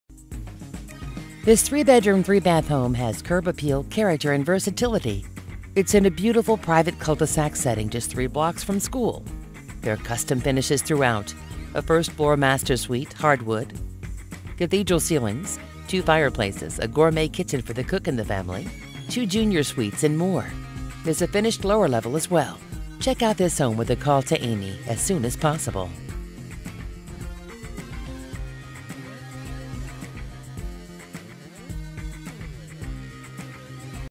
812 Yale Ave Terrace Park, OH | MLS# 1524204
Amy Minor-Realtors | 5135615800

Curb appeal, Character and versatility! Beautiful, private culdesac setting, 3 blocks from school. custom finishes, 1st floor master suite, hwd, cathedral ceilings, 2 FP's, gourmet kit, finished LL, two junior suites and more! Best Buy in Terrace Park